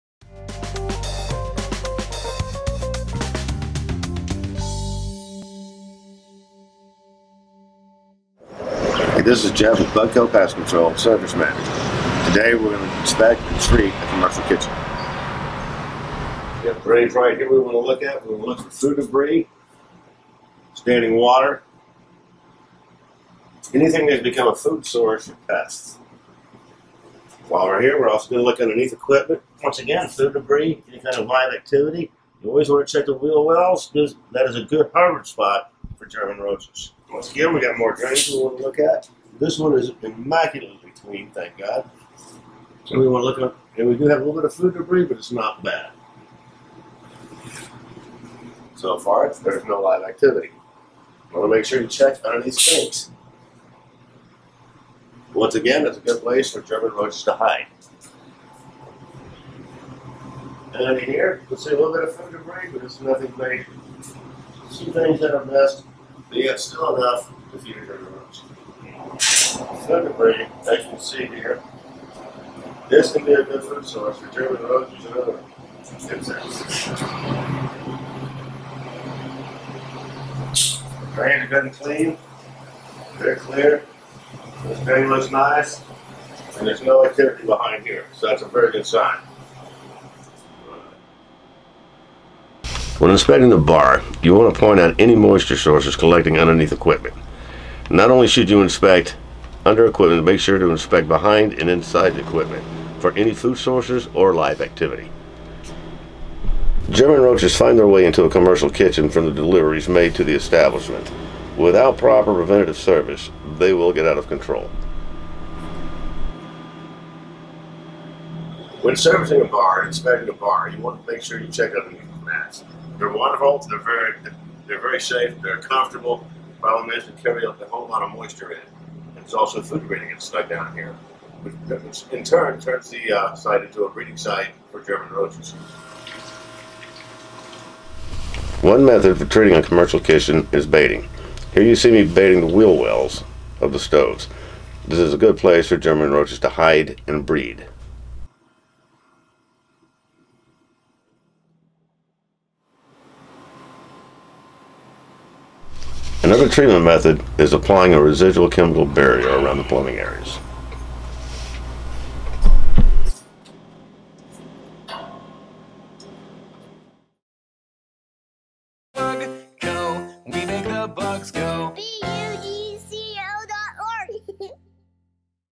How to Prevent Pests in Commercial Kitchens: Insights from Pest Control Technicians
Keeping your commercial kitchen pest-free is essential for both food safety and maintaining a clean reputation. In this video, pest control experts share their top insights on how to prevent pests in your commercial kitchen. From identifying common entry points to implementing proper sanitation practices, we cover the key areas pest control technicians focus on during an inspection. Learn how to create an effective pest prevention plan, avoid costly infestations, and ensure your kitchen stays up to code with health and safety standards.

👨‍🍳 Ensure a safe, pest-free environment in your kitchen by following these pro tips! 🍽️